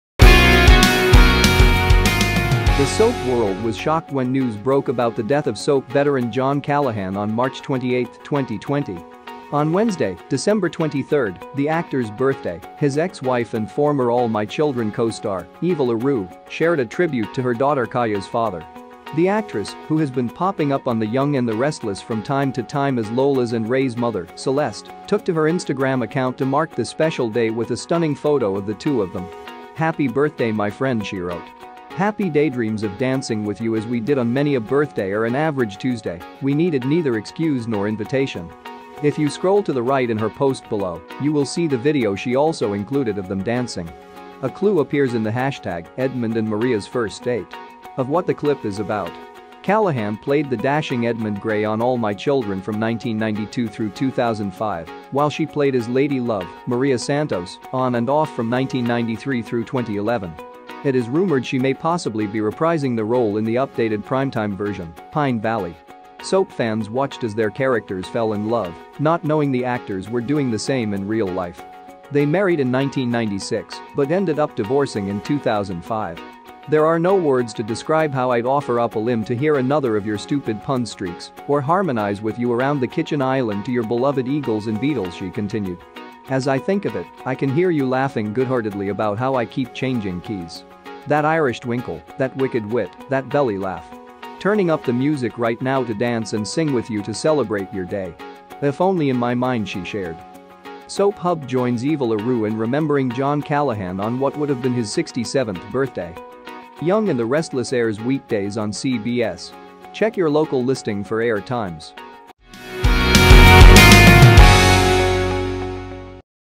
Soap Veteran Eva LaRue Remembers John Callahan On His Birthday
The soap world was shocked when news broke about the death of soap veteran John Callahan on March 28, 2020. On Wednesday, December 23, the actor's birthday, his ex-wife and former All My Children co-star, Eva LaRue, shared a tribute to her daughter Kaya's father.